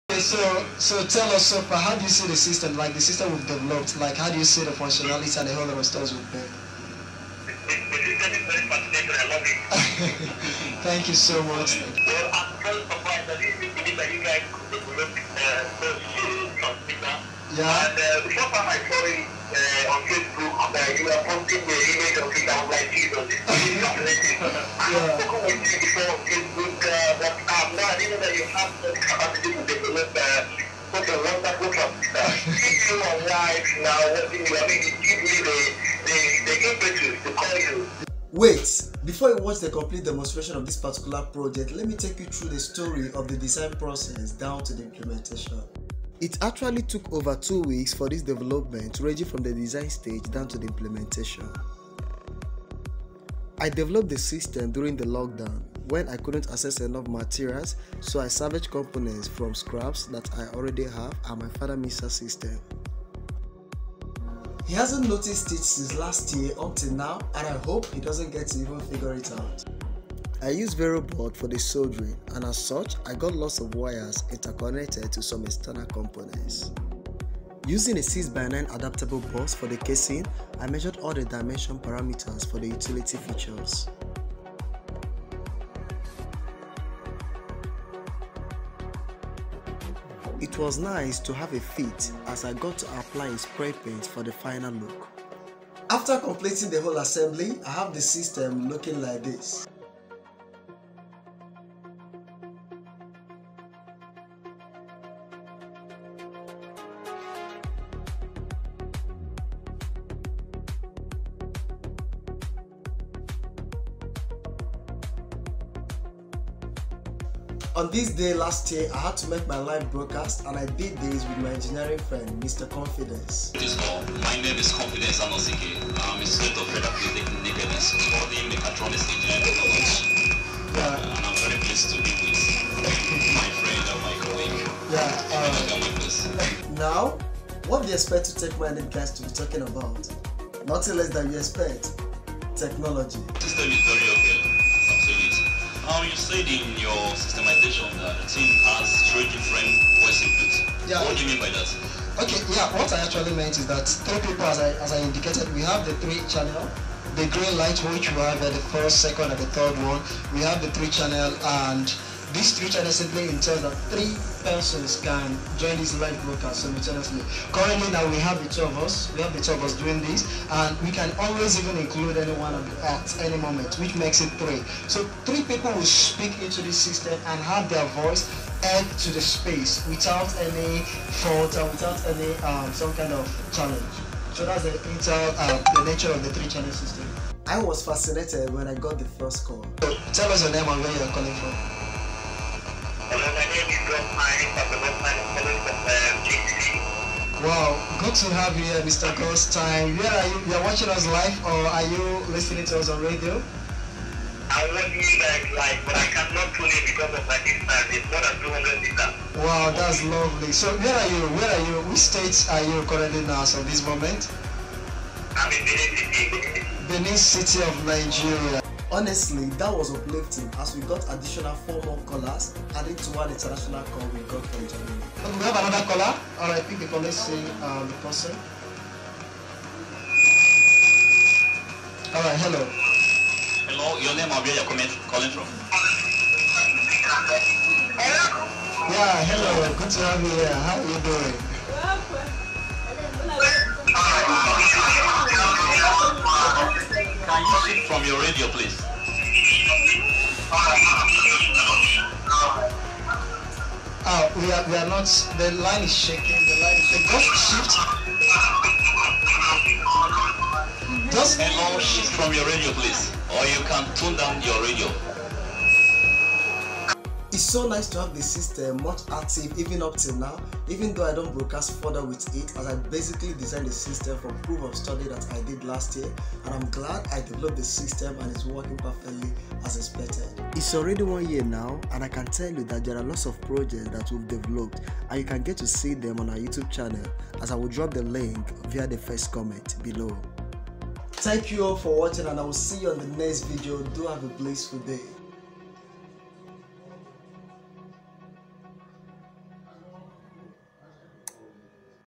My best Moments Broadcasting with my local made FM Radio Transmitter
In this video, I will take you through the story of how I designed and develop the 3 channel audio mixer which I further Integrated with an FM radio transmitter.